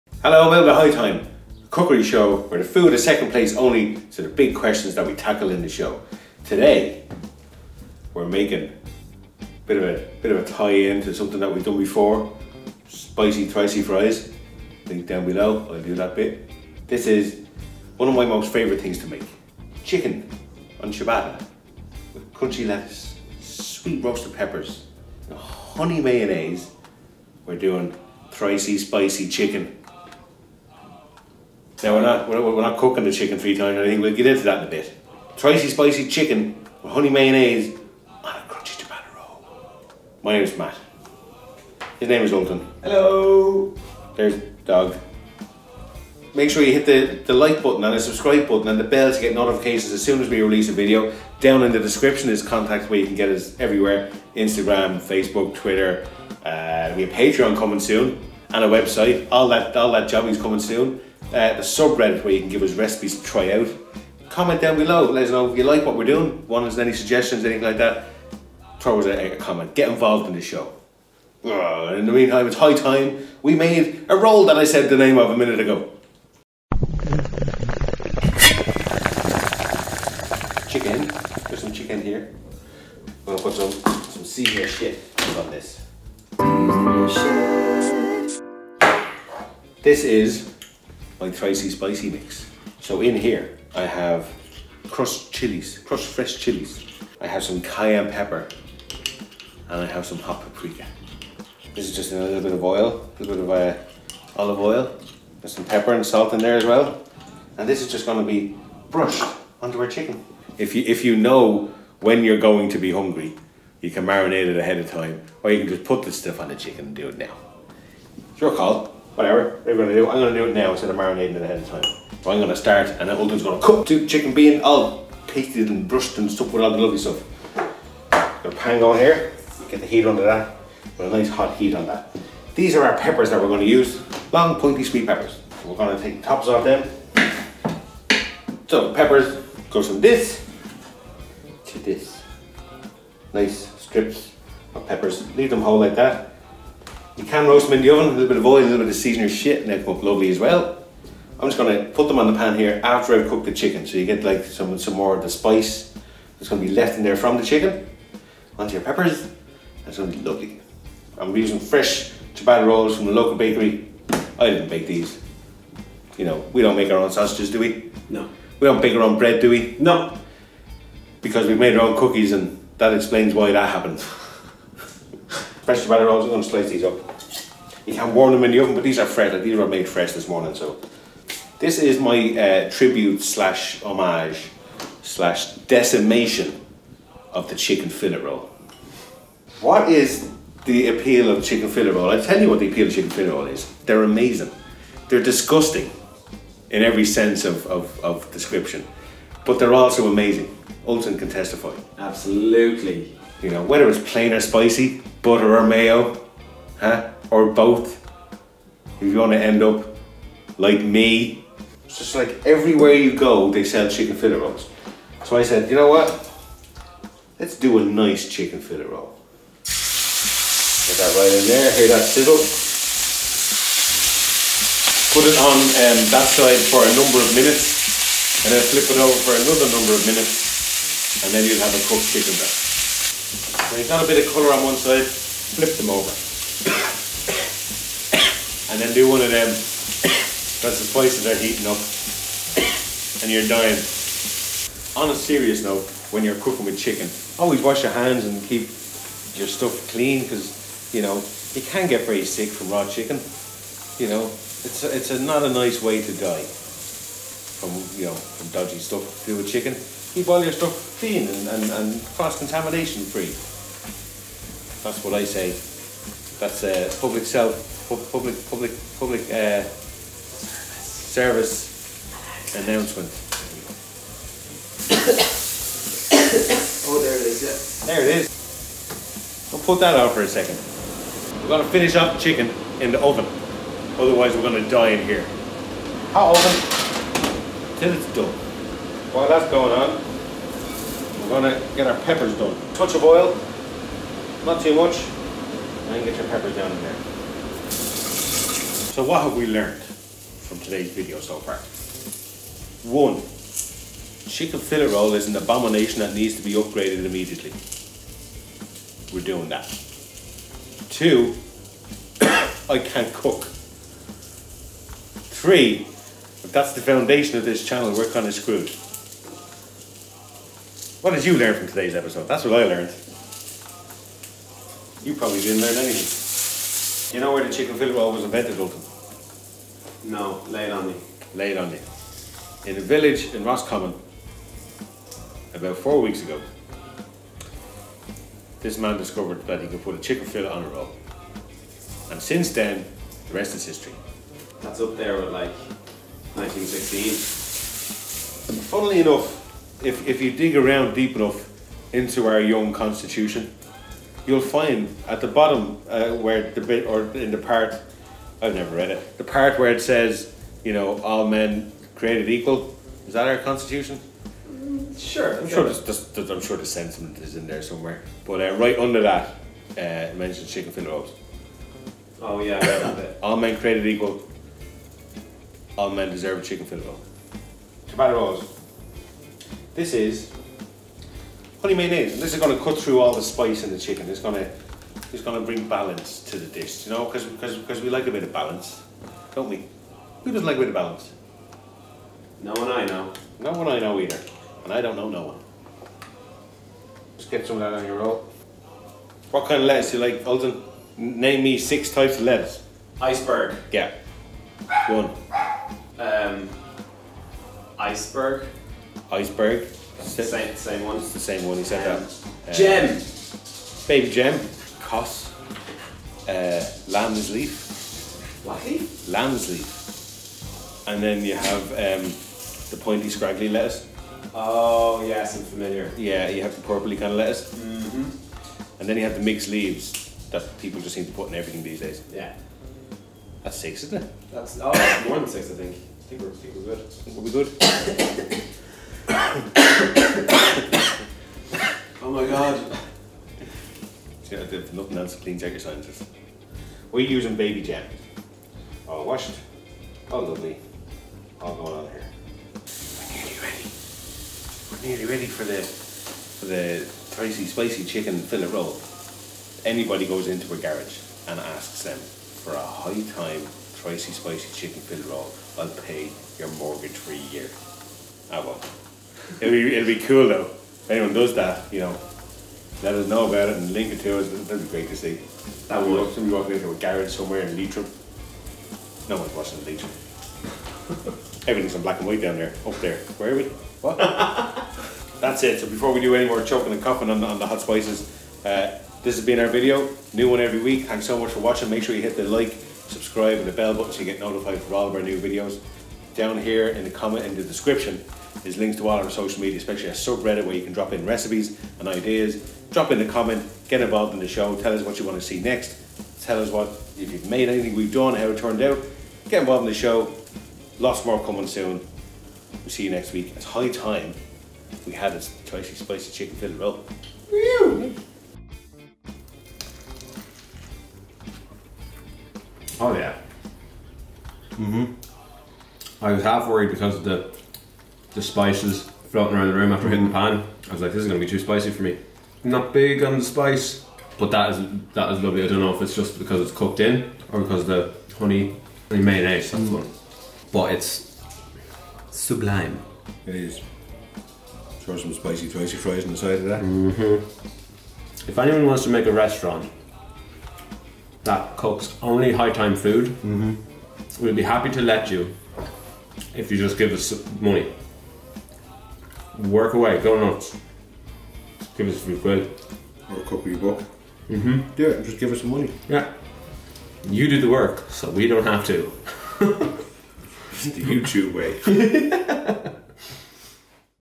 THRICEY SPICY CHICKEN FILLET ROLL - High Time Episode #16
A chicken fillet roll to right all of the wrongs done by the shoddy corner shop chicken fillet rolls that came before it!

Why not have some SPICY THRICEY FRIES on the side?

What should we make next?  Let us know below and we'll give it a go!

You can also check out Matt's podcast Pod Skwod here (it's for grown ups only) -

As always, don't forget to always